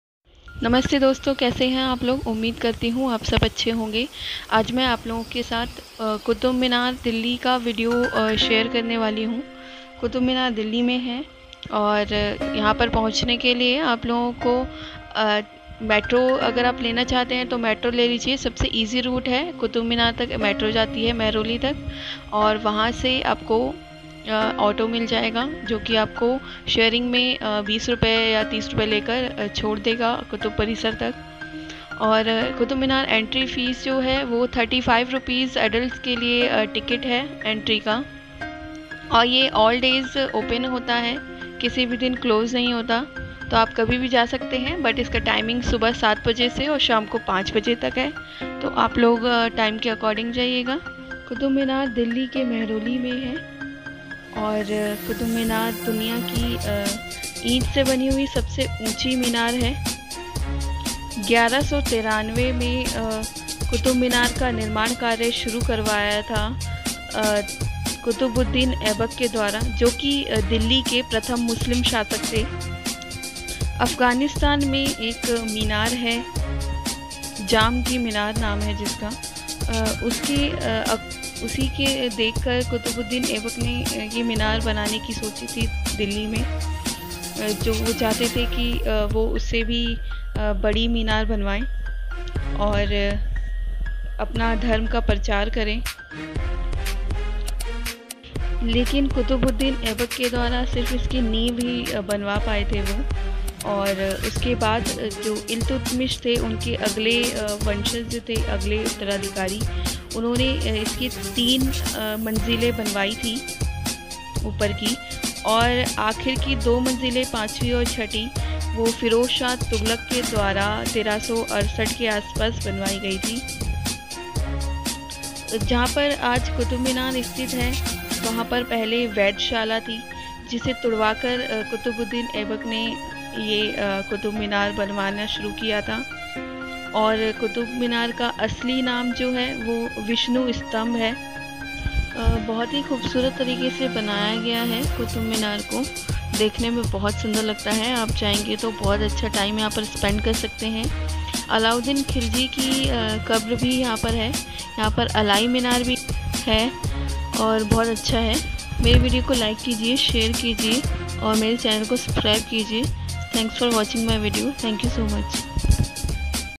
QUTUB MINAR Delhi/ Qutab Minar India/ Qutub Parisar Mehrauli Delhi, The Tallest Tower in The World
Qutub Minar Duniya ki Sabse Unchi MINAR Hai jo ki Bricks Se Banai Gayi Hai. Iska Nirman Karya 1193 me Qutubuddin Ebak ne Start Karwaya tha par wo iski sirf adhaar hi banwa paya tha uske baad iltutmish ne iski 3 Manjila banwai or baki ki 2 Manjile Firozshah tugalak ne banwai thi 1368 mai. Qutub minar ko Qutab minar bhi kaha jata hai par iska asli naam Vishnu Stambh hai kyunki ye pehle ek vedhshala thi jise tudavakar Qutubuddin ne qutub minar ka nirman start karwaya tha.